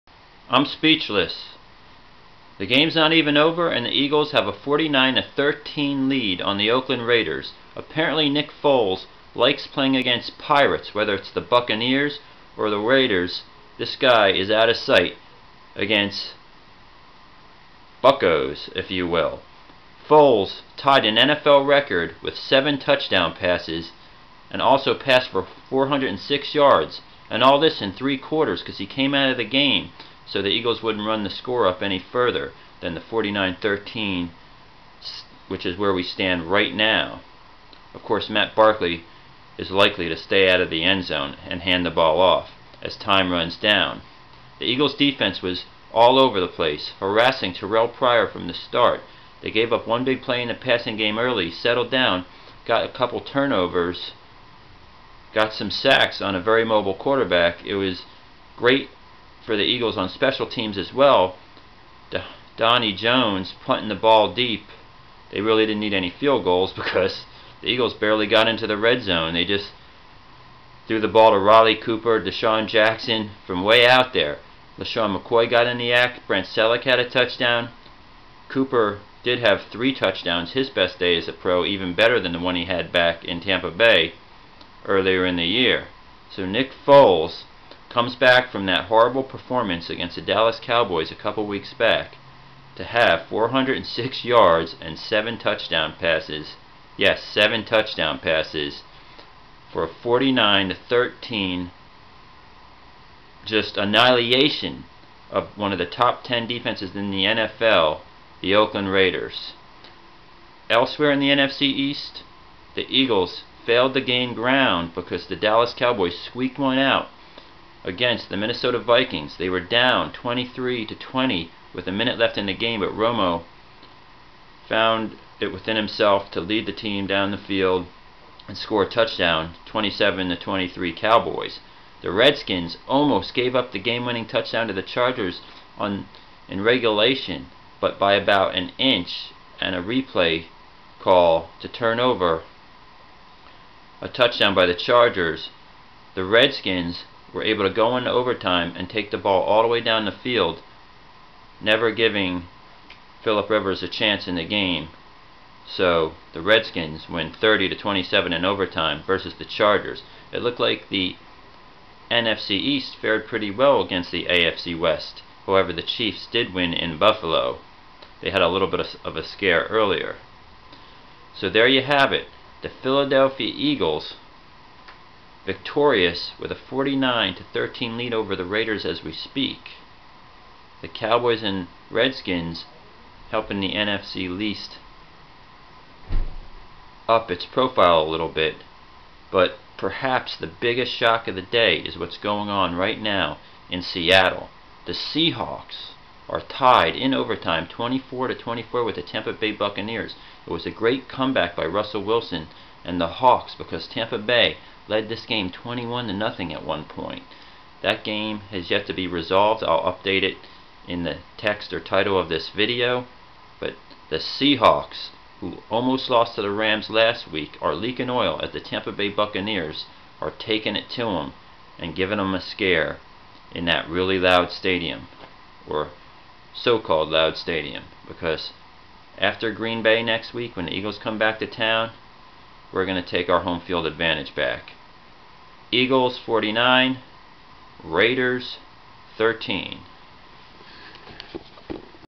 Nick Foles 7 TD Passes Lead Eagles Past Raiders 49-20: 406 Yards For QB !
Riley Cooper 3 TD: Desean Jackson, Mccoy, Celek, all score